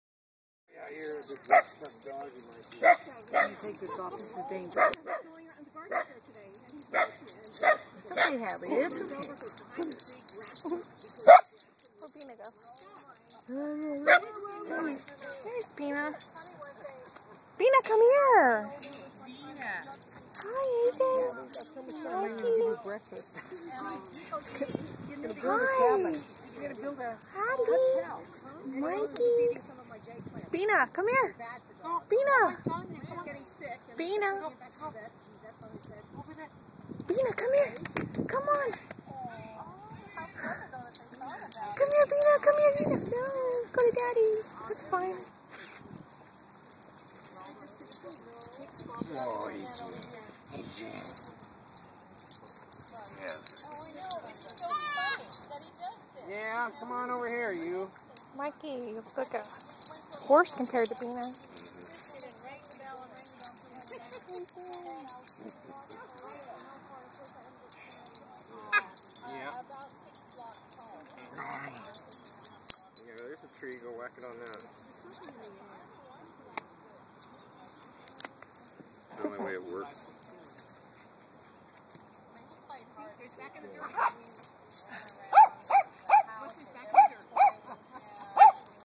Italian Greyhounds at Alice's dog park
Italian Greyhounds Mike, Hazen, Bina and Hadley at Alice's Dog Park.